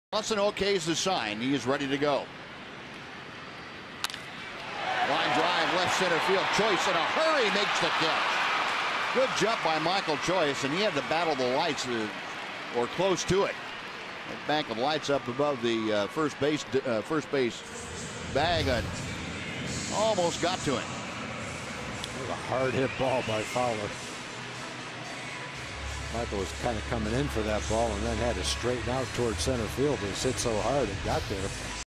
TEX@HOU: Choice chases down a line drive in left
8/30/14: Michael Choice tracks down a line drive and makes a nice grab in left field, taking an extra-base hit away from Dexter Fowler